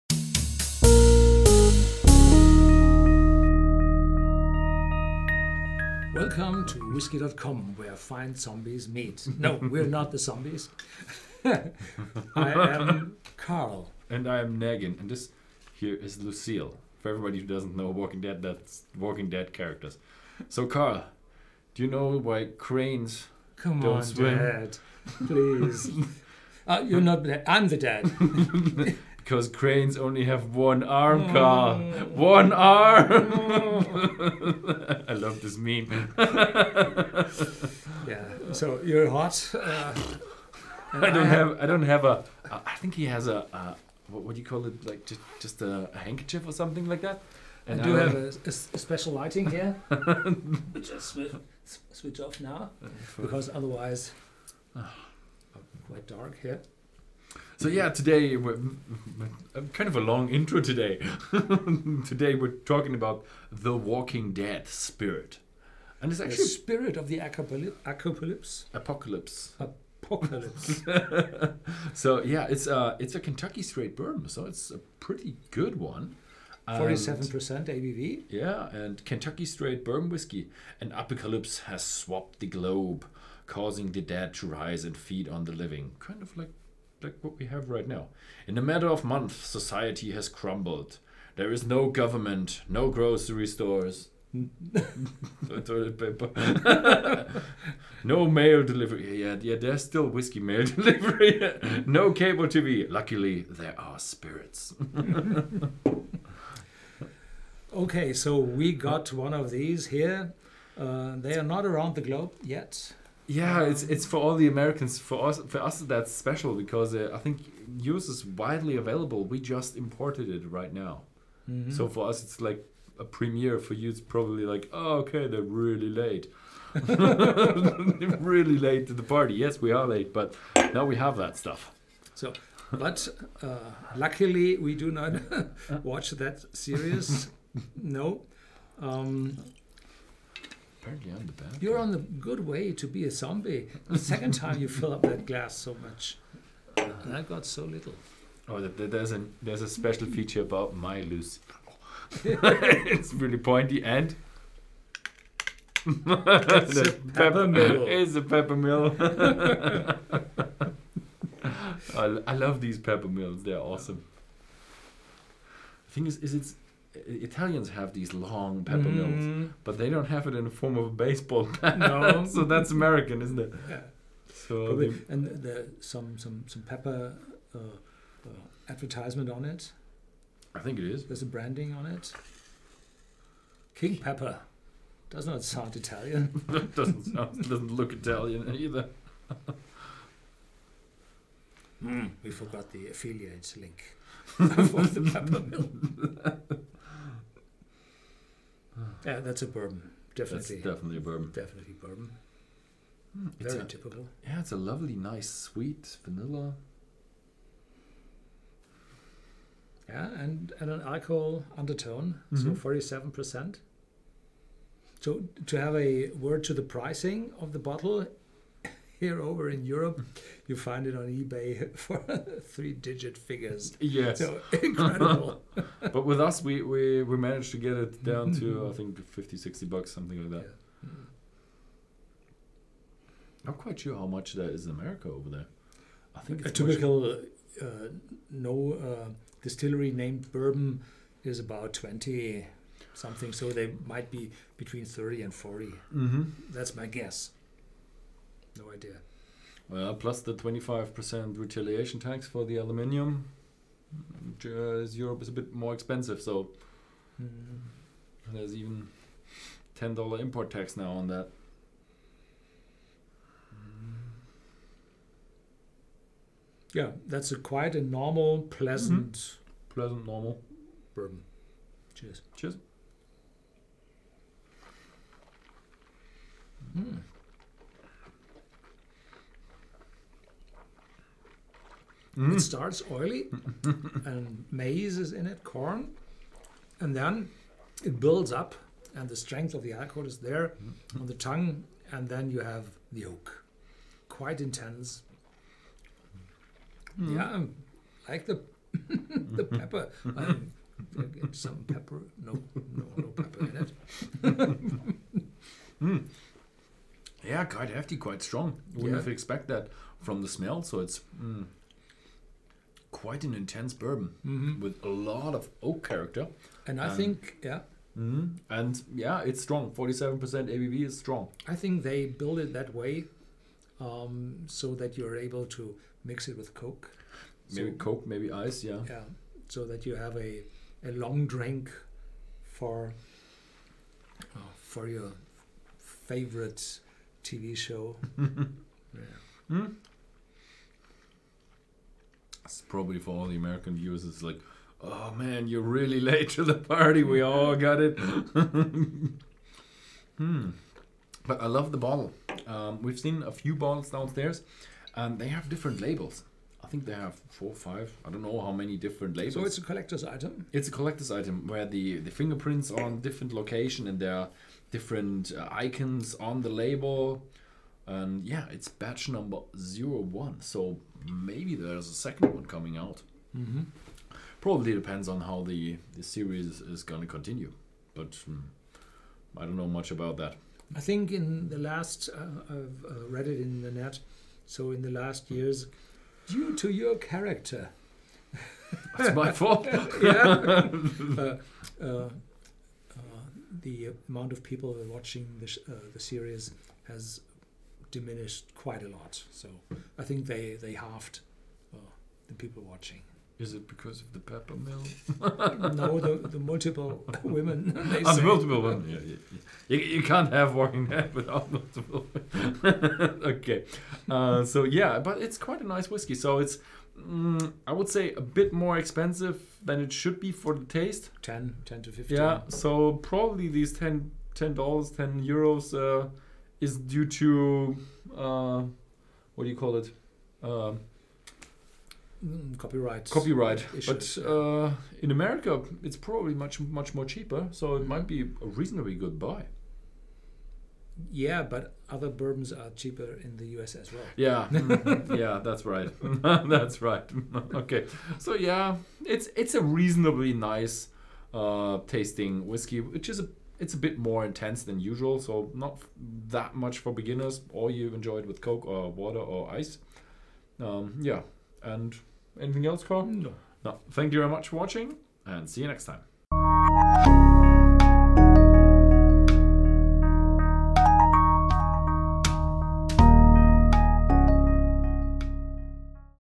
The Walking Dead - Spirits of the Apocalypse
Nosing 03:07

Whisky.com reviews the The Walking Dead - Spirits of the Apocalypse

whisky whiskey # Visit our online shop at Whisky.com